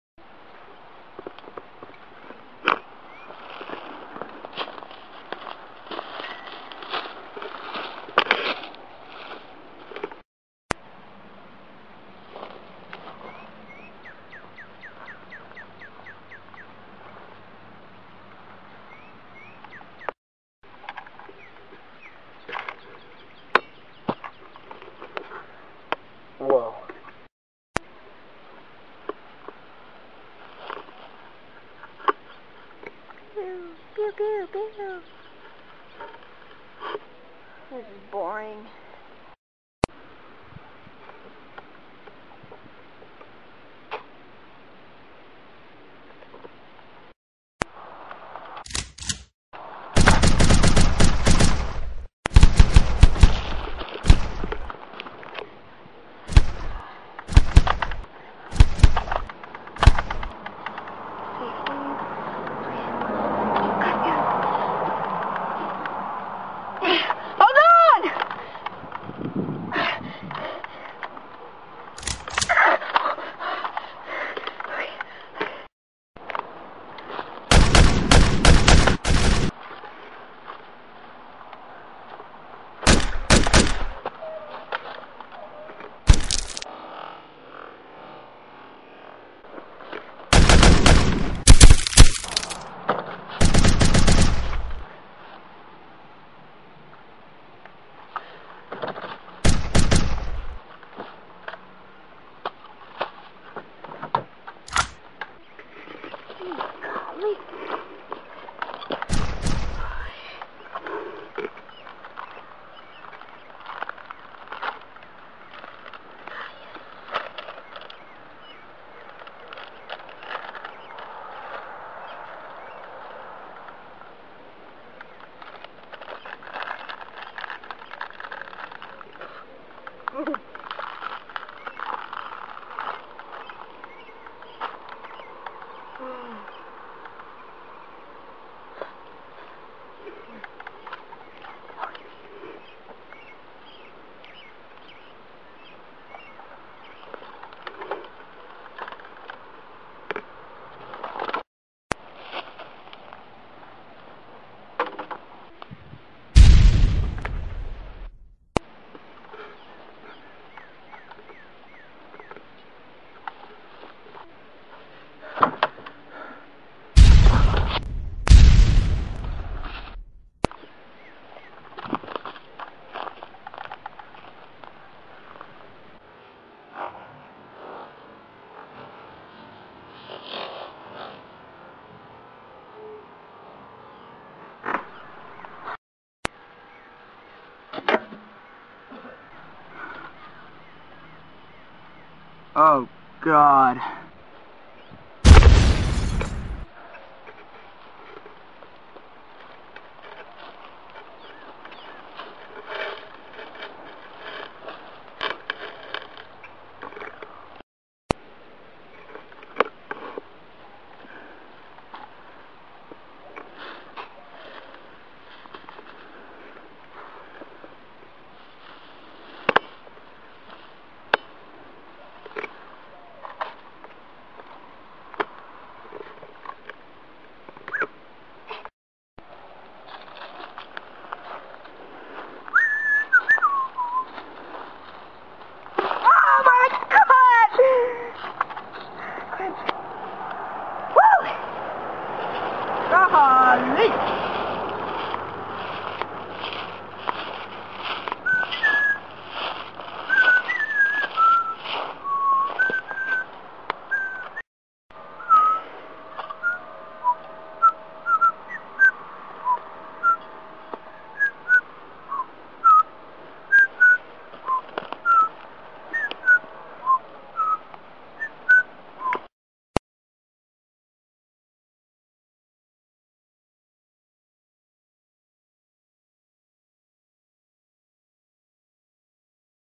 Creek Shoot Out
two crazy kids decide to fight it out in a clearing and one ends up dying and the other looses his golf cart :( foolish childern favorite and like it!